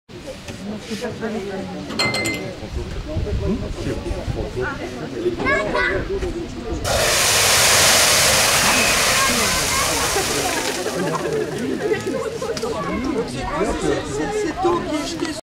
pompiers
extinction friteuse en feu (pompiers estaimpuis)